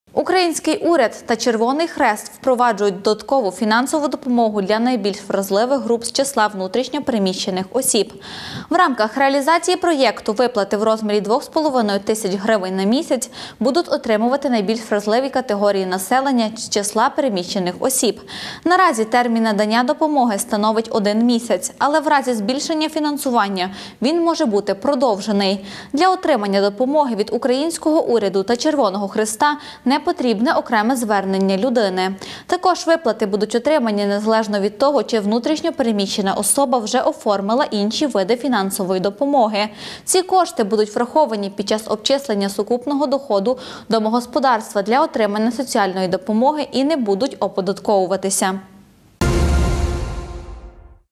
Додаткова допомога для біженців
Український уряд та Червоний Хрест впроваджують додаткову фінансову допомогу для найбільш вразливих груп з числа внутрішньо переселених осіб.  В рамках реалізації проєкту виплати в розмірі 2500 гривень на місяць будуть отримувати найбільш вразливі категорії населення з числа внутрішньо переміщенних осіб. Наразі термін надання допомоги становить 1 місяць, але в разі збільшення фінансування він може бути продовжений.  Для отримання допомоги від українського уряду та Червого хреста не потрібне окреме звернення людини. Також виплати будуть отримані незалежно від того, чи внутрішньо переміщена особа вже оформила інші види фінансової допомоги. Ці кошти не будуть враховані під час обчислення сукупного дохода домогосподарства для отримання соціальної допомоги і не будуть оподатковуватися.